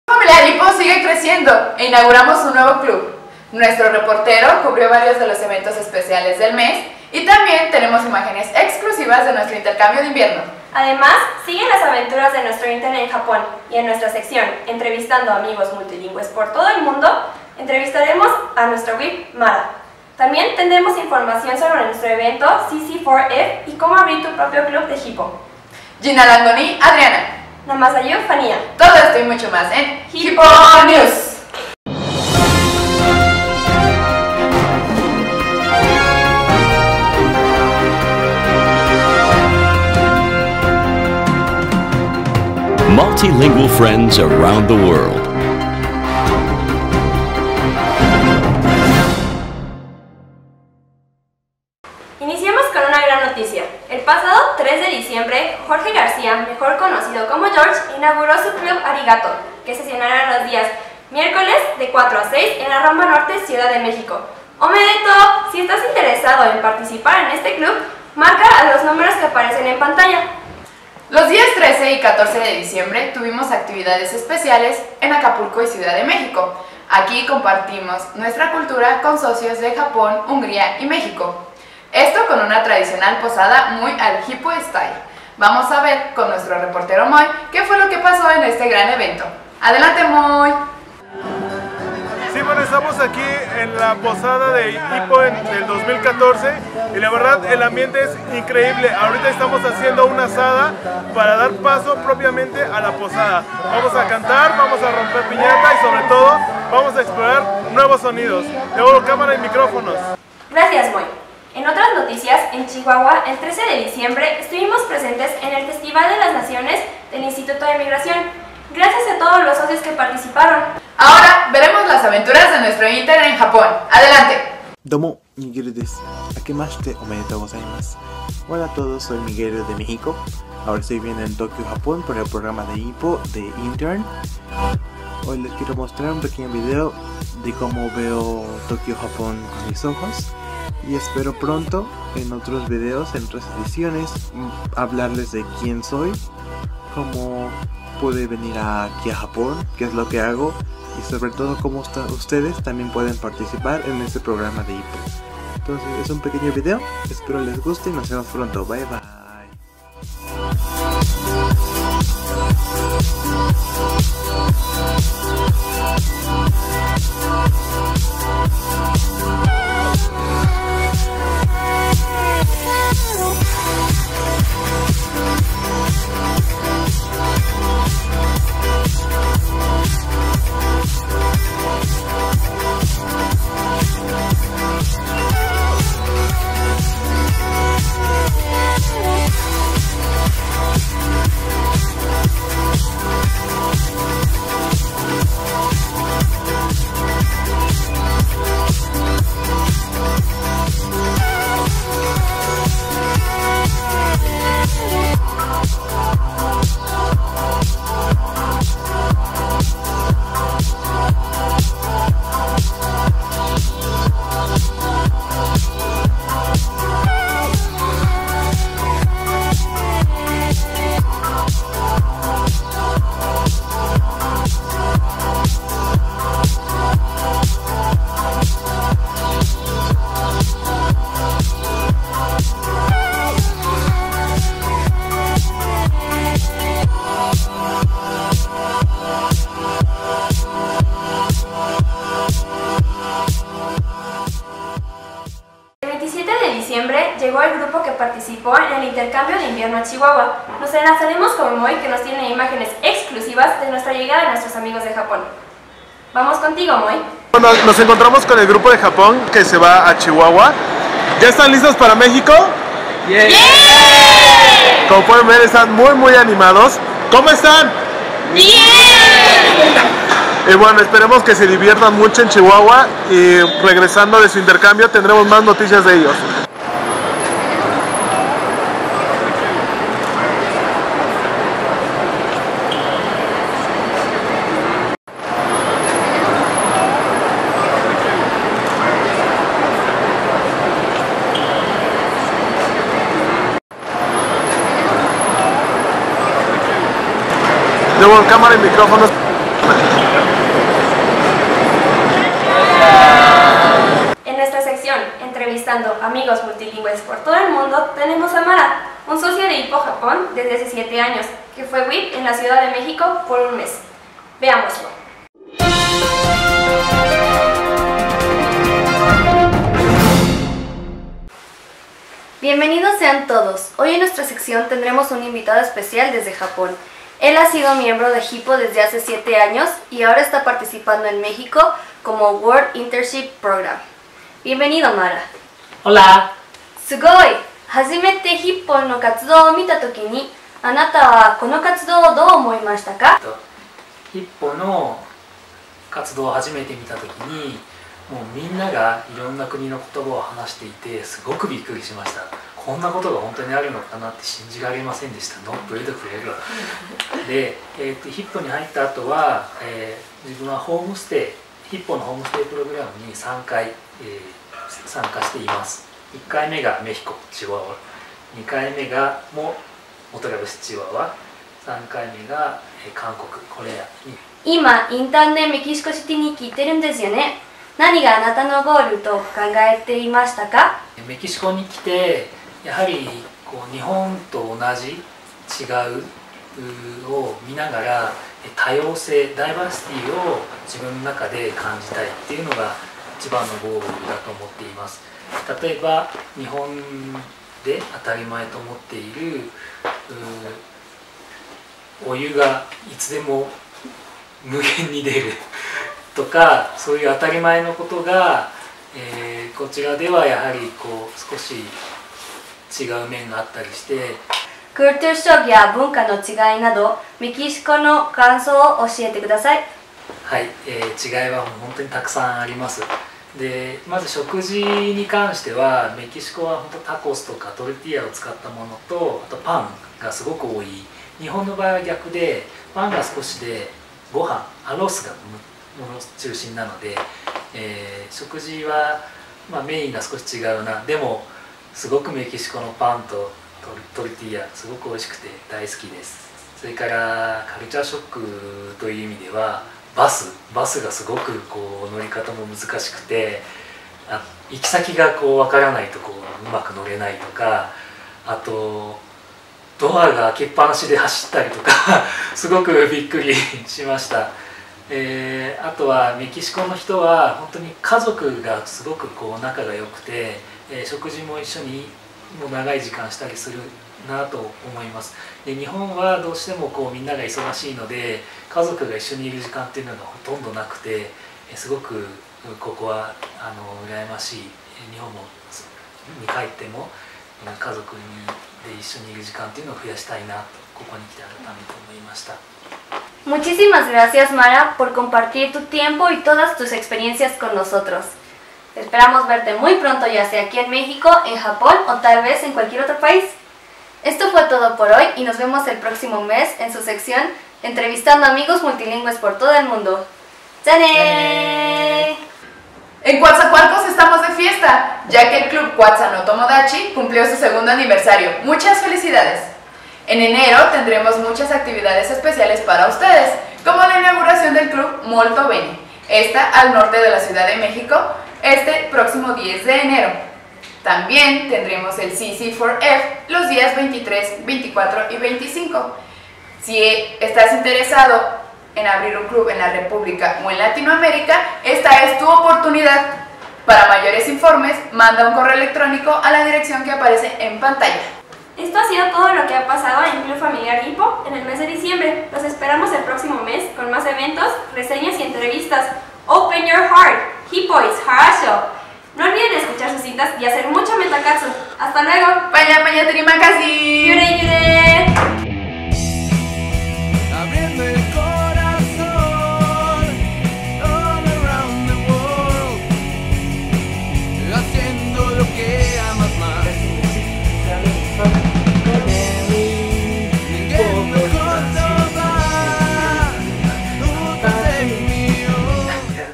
Hippo News Diciembre 2014
Noticias de todos los eventos e intercambios de Club Familiar Hippo en el mes de Diciembre.